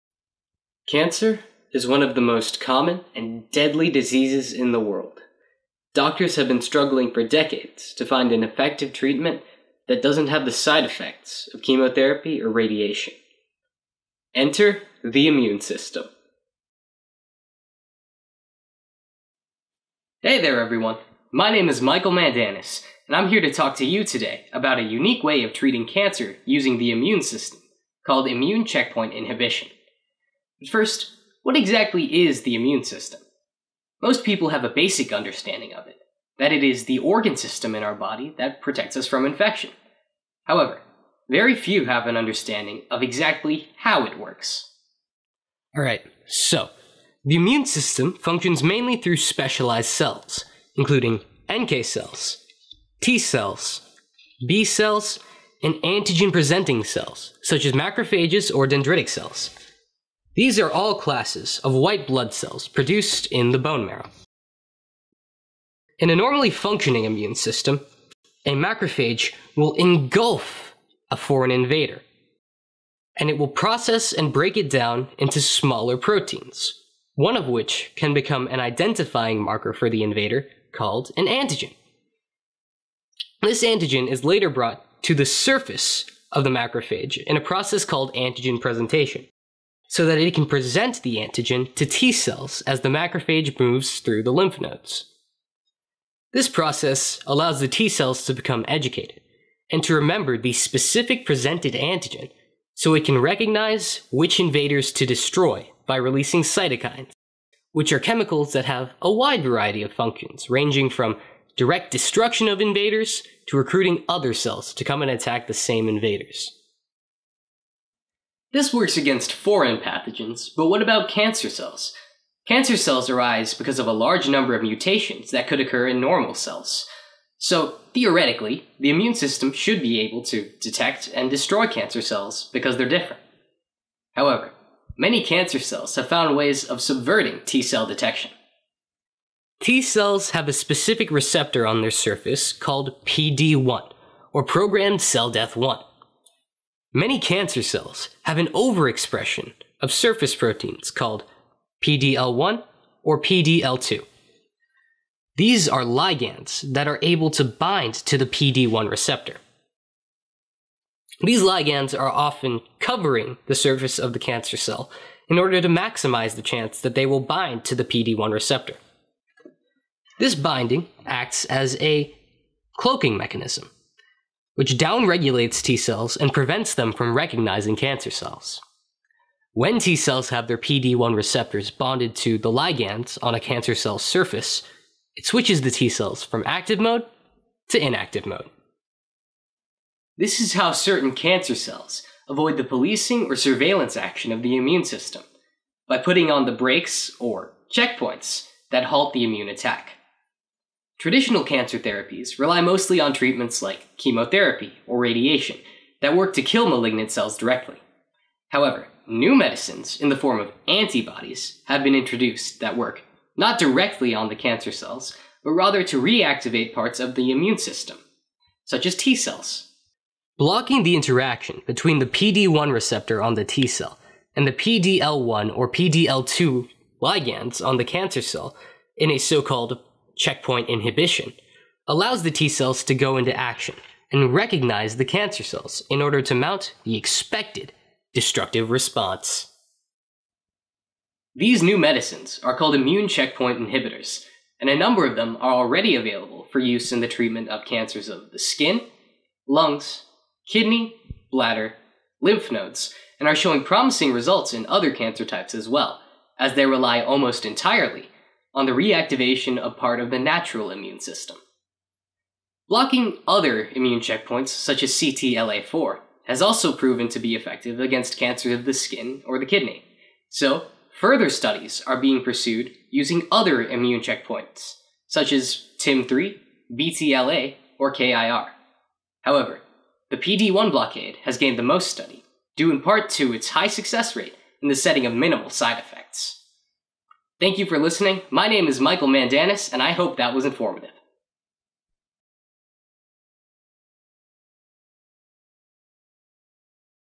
The Immune System: Friend and Foe - Immune Checkpoint Inhibitors - Michael Mandanas
A brief, simple explanation of how immune checkpoint inhibitors work to fight cancer. Delivered by Michael Mandanas (15 yo).